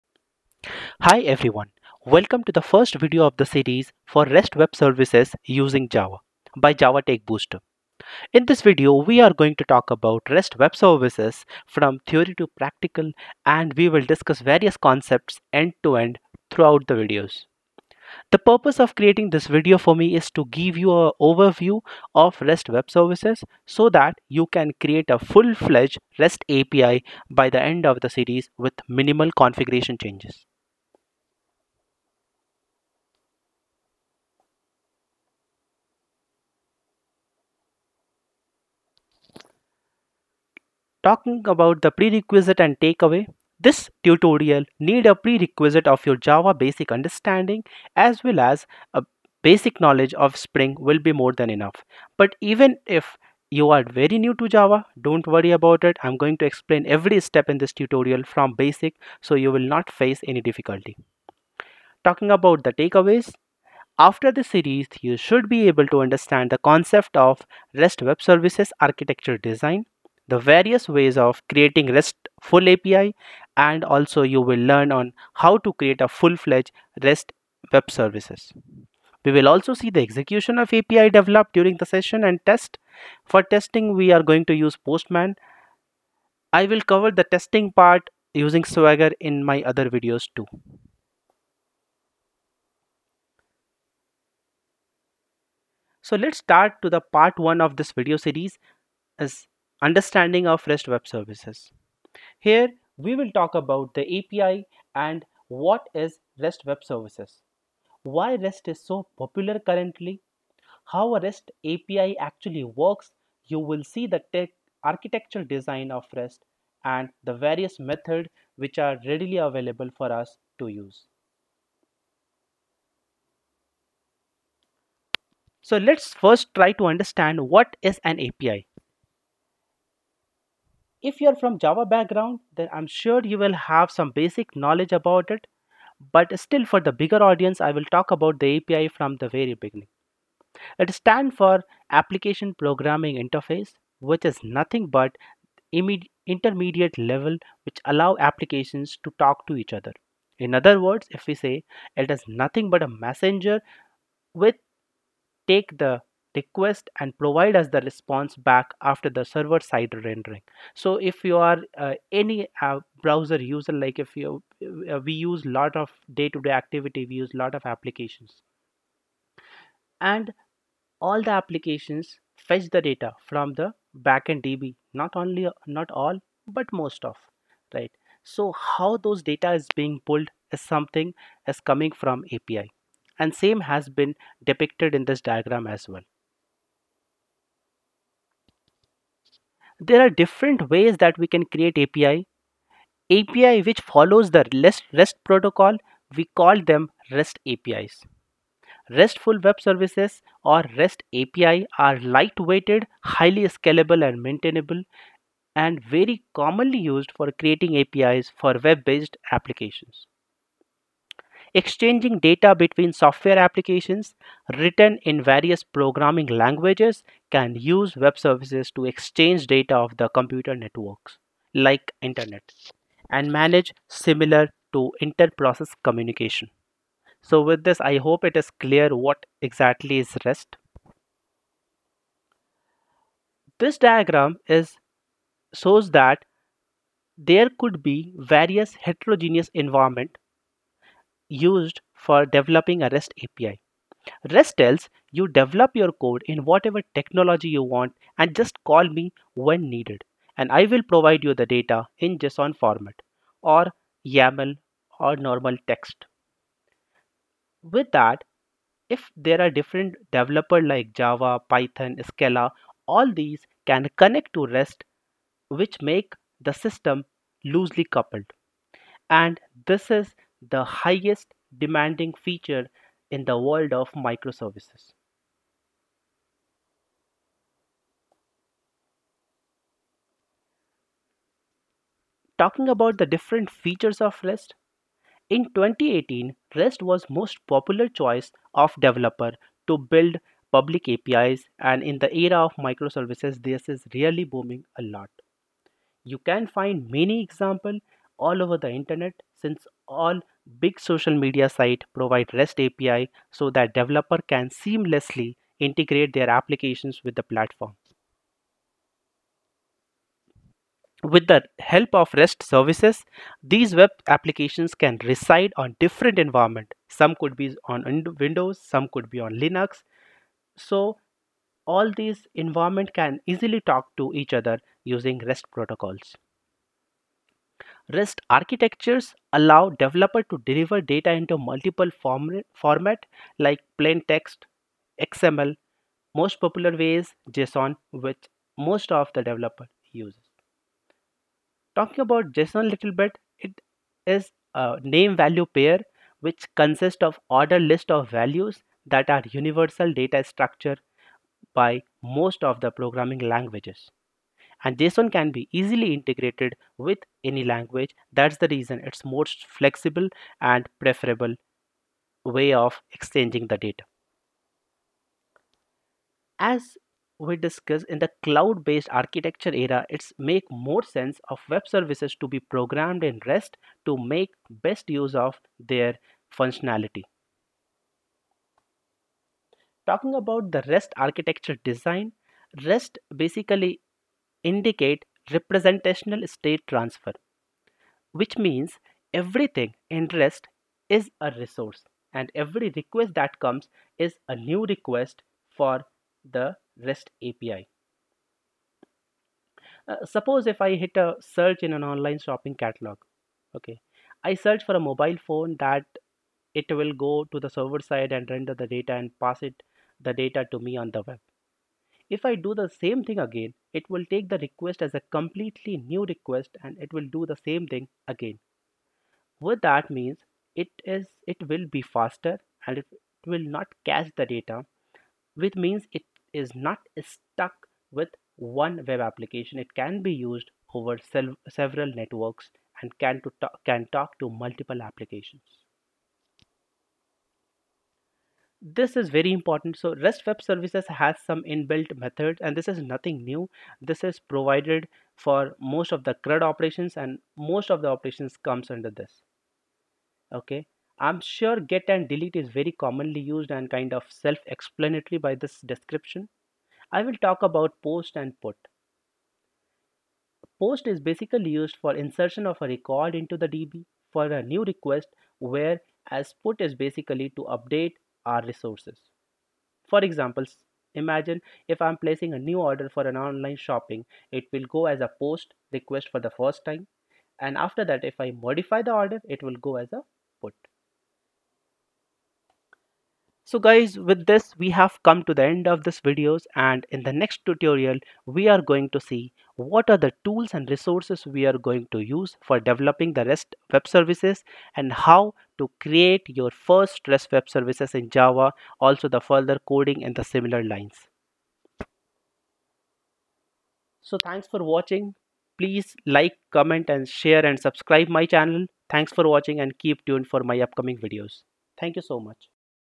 What is REST API? || Rest API Tutorial ||REST API Concepts and Examples|| Rest Web Services Tutorial
This video is to discuss about REST API concepts and examples, REST use case & REST Architecture.

Suitable for those who want to understand REST API with minimal effort.

Suitable for both Java and Non Java Folks.